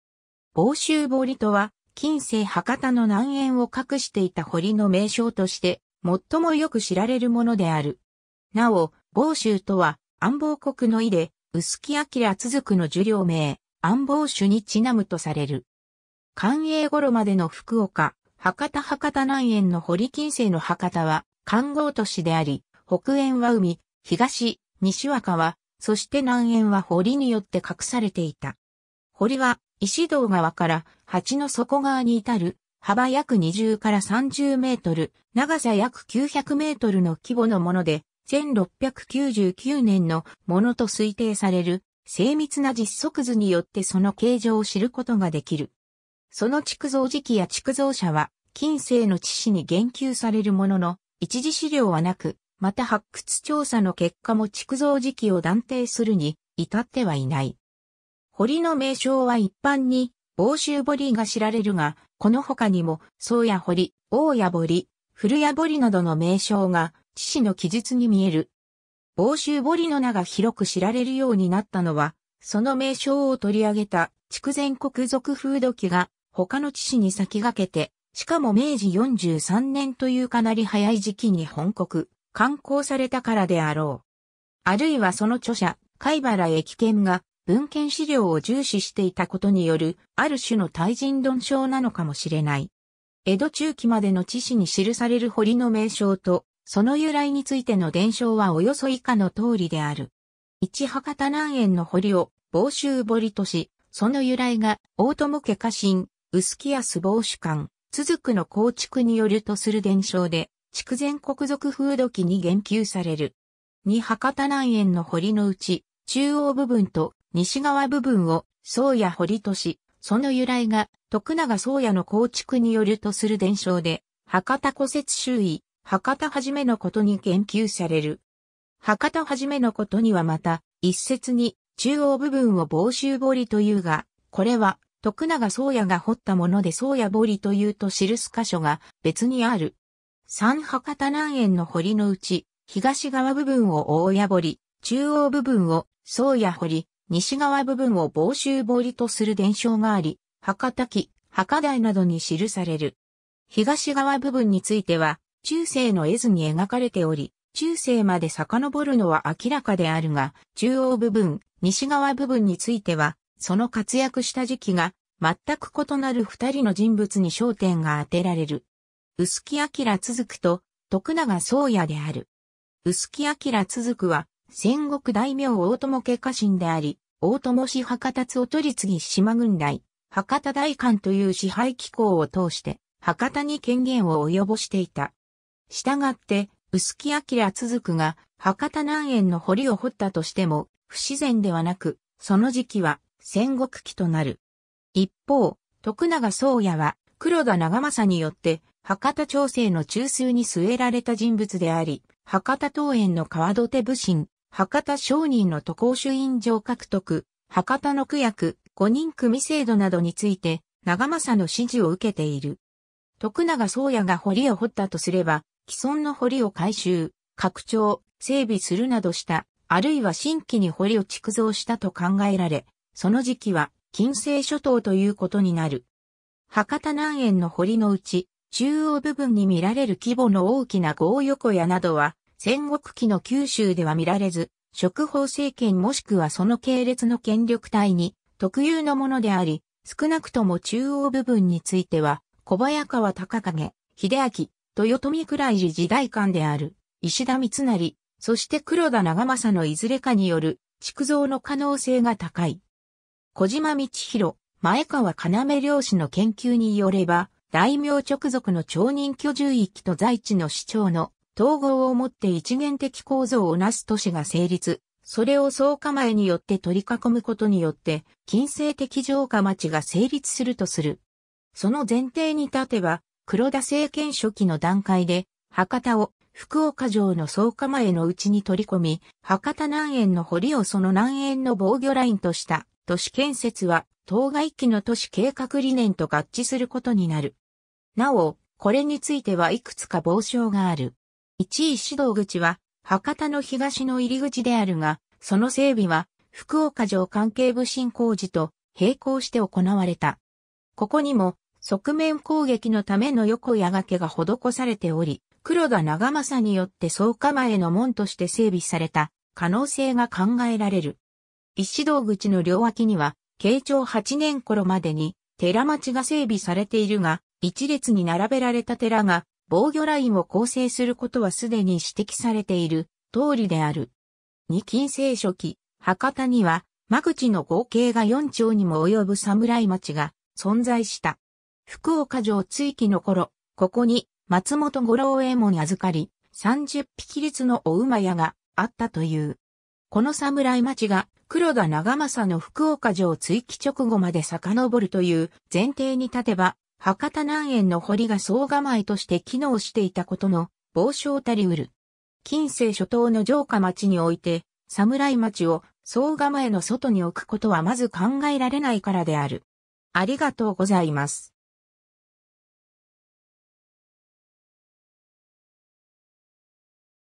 房州堀
房州堀（ぼうしゅうぼり）とは、近世博多（筑前国・那珂郡）の南縁を画していた堀の名称として最もよく知られるものである。
なお、「房州」とは「安房国」の意で、臼杵鑑続（後述）の受領名「安房守」にちなむとされる。
 寛永ごろまでの福岡・博多（推定） 博多南縁の堀 近世の博多は環濠都市であり、北縁は海、東・西は川、そして南縁は堀によって画されていた。
堀は石堂川から鉢の底川（那珂川の支流）に至る、幅約20～30m、長さ約900mの規模のもので、1699年のものと推定される精密な実測図によってその形状を知ることができる。
その築造時期や築造者は、近世の地誌に言及されるものの、一次史料はなく、また発掘調査の結果も築造時期を断定するに至ってはいない。
 堀の名称は一般に「房州堀」が知られるが、このほかにも宗也堀、大屋堀（太屋堀）、古屋堀などの名称が地誌の記述に見える。
「房州堀」の名が広く知られるようになったのは、その名称を取り上げた「筑前国続風土記」が、他の地誌に先駆けて、しかも明治43(1910)年というかなり早い時期に翻刻・刊行されたからであろう。
あるいはその著者、貝原益軒が文献史料を重視していたことによるある種の対人論証なのかもしれない。
 江戸中期までの地誌に記される堀の名称とその由来についての伝承はおよそ以下の通りである。
 1）博多南縁の堀を「房州堀」とし、その由来が大友家家臣、臼杵安房守鑑続の構築によるとする伝承で、「筑前国続風土記」(1703年) に言及される。
 2）博多南縁の堀のうち、中央部分と西側部分を「宗也堀」とし、その由来が徳永宗也の構築によるとする伝承で、「博多古説拾遺」(1736年) 「博多始之事」(1747年)に言及される。
「博多始之事」にはまた、「一説に中央部分を房州堀というが、これは徳永宗也が掘ったもので宗也堀という」と記す箇所が別にある。
 3）博多南縁の堀のうち、東側部分を大屋（フトヤ）堀、中央部分を宗也堀（あるいは徳永宗也が掘った堀）、西側部分（あるいは中央部分から西側部分にかけて）を房州堀とする伝承があり、「博多記」(1723年)、「覇家台」などに記される。
 東側部分については、中世の絵図に描かれており、中世まで遡るのは明らかであるが、中央部分・西側部分については、その活躍した時期が全く異なる二人の人物に焦点があてられる。
臼杵鑑続と徳永宗也である。
臼杵鑑続は戦国大名大友家家臣であり、大友氏－博多津御取次（加判衆）－志摩郡代－博多代官という支配機構を通して博多に権限を及ぼしていた。
したがって、臼杵鑑続が博多南縁の堀を掘ったとしても不自然ではなく、その時期は戦国期となる。
一方、徳永宗也は黒田長政によって博多町政の中枢に据えられた人物であり、博多東縁の川土手普請、博多商人の渡航朱印状獲得、博多の公役・五人組制度などについて長政の指示を受けている。
徳永宗也が堀を掘ったとすれば、既存の堀を改修・拡張・整備するなどした、あるいは新規に堀を築造したと考えられ、その時期は近世初頭ということになる。
 博多南縁の堀のうち、中央部分に見られる規模の大きな合横矢（虎口両側からの側面攻撃を可能にする）などは、戦国期の九州では見られず、織豊政権もしくはその系列の権力体に特有のものであり、少なくとも中央部分については小早川隆景・秀秋、豊臣蔵入地代官である石田三成、そして黒田長政のいずれかによる築造の可能性が高い。
 小島道裕・前川要両氏の研究によれば、大名直属の町人居住域と在地の市町の統合をもって一元的構造をなす都市が成立、それを惣構によって取り囲むことによって近世的城下町が成立するとする。
その前提に立てば、黒田政権初期の段階で、博多を福岡城の惣構の内に取り込み、博多南縁の堀をその南縁の防禦ラインとした都市建設は、当該期の都市計画理念と合致することになる。
なお、これについてはいくつか傍証がある。
 1）石堂口は博多の東の入口であるが、その整備は福岡城関係普請工事と平行して行なわれた。
ここにも側面攻撃のための横矢がけが施されており、黒田長政によって惣構の門として整備された可能性が考えられる。
石堂口の両脇には慶長8年頃までに寺町が整備されているが、一列に並べられた寺が防禦ラインを構成することは既に指摘されている通りである。
 2）近世初期（1643年）、博多には間口の合計が四町にも及ぶ侍町が存在した。
福岡城築城の頃、ここに「松本五郎右衛門預かり、三十疋立の御厩」があったという。
この侍町が黒田長政の福岡城築城...